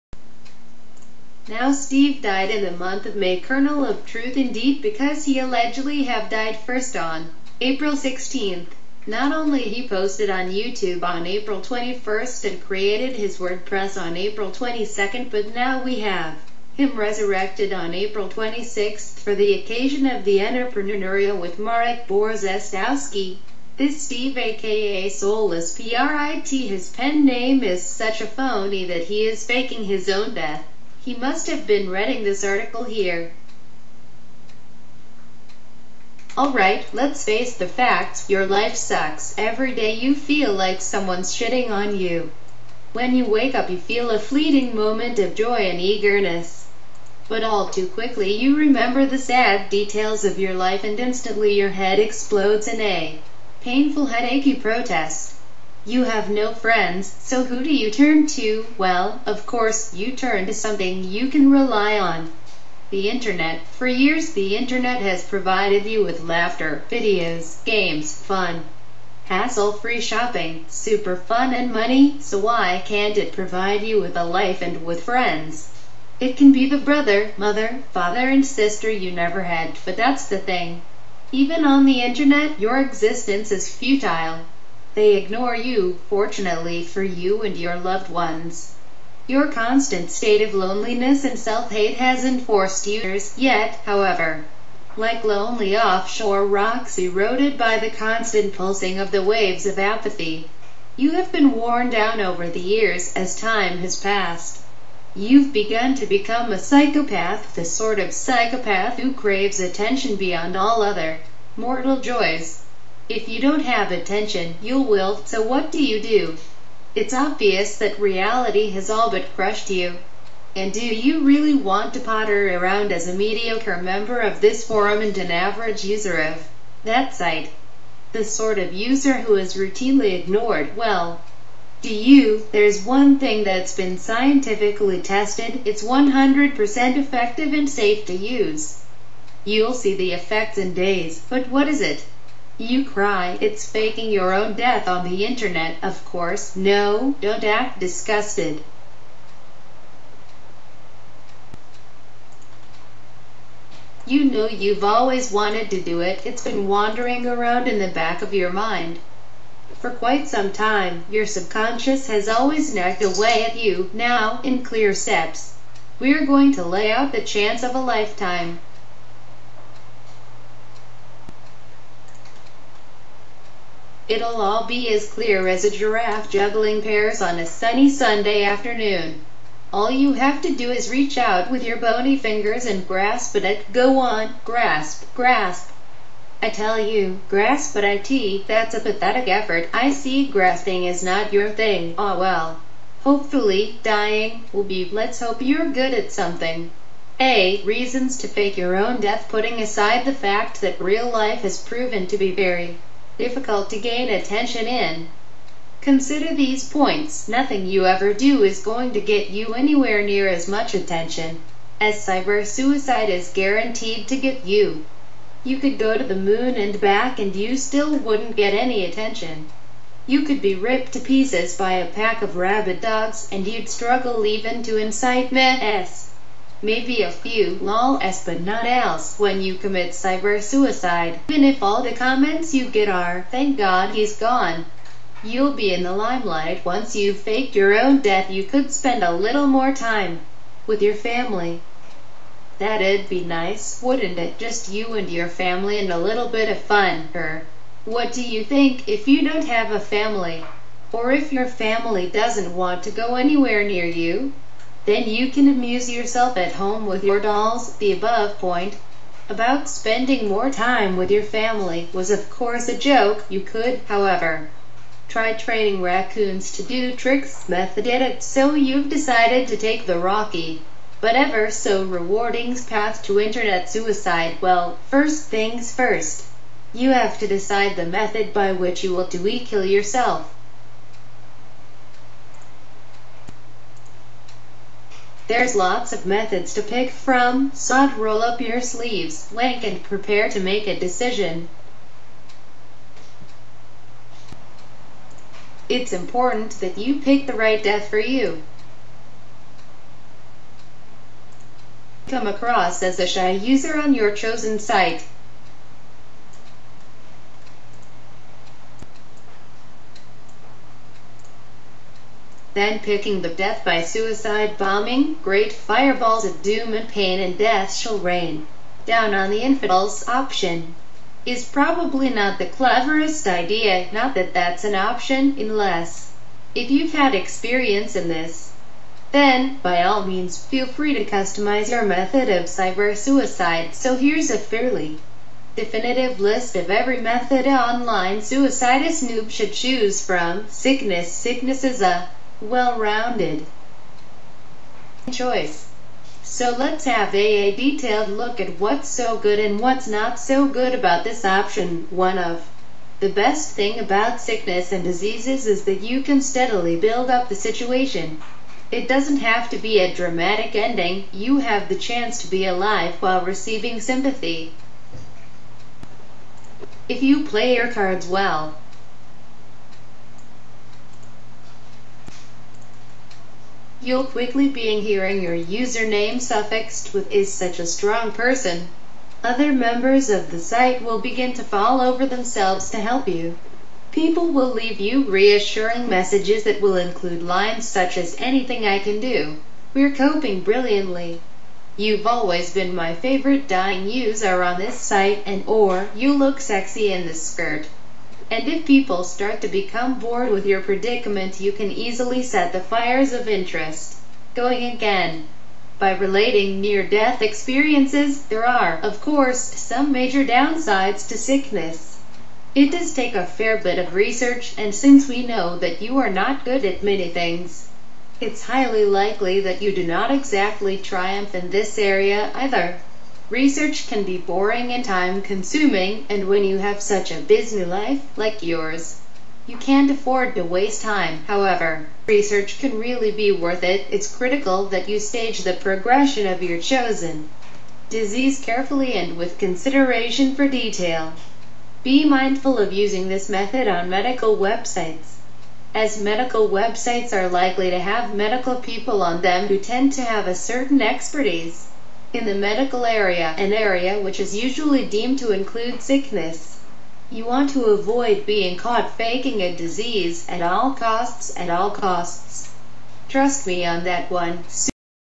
SLEAZIN' RISK SICKNESS IS FAKING HIS DEATH WITH THE HELP OF ''SOUL-LESS-PEE'' OR ''SOIL-AS-PEE''
STEVE SYDNESS, THE KING OF HOAXES
"Crash the Teaparty" website hosted by former GOP Senate candidate Steve Sydness THE KING OF HOAX

Now Steve died in the month of May KERNEL OF TRUTH INDEED
because he allegedly have died first on April 16th.
THERE ARE NO FAMILY OBITUARIES BTW
Not only he posted on YouTube on April 21st and created his WordPress on April 22nd,
now we have him resurrected on April 26th for the occasion of the entrepreneuria with Marek Borzestowski.

This Steve aka Soulless-Prit is such a phoney that he is faking his own death.

If Steve Sydness died on April 16th, then how did he upload this video on his channel on April 21st

YOU PROVED MANY TIMES THAT YOU ARE NOT A PERSON. YOU ARE SOULLESS-PRIT ENTITY SO GET THAT CRAZY IDEA OUT OF WHAT IS LEFT OF YOUR MIND..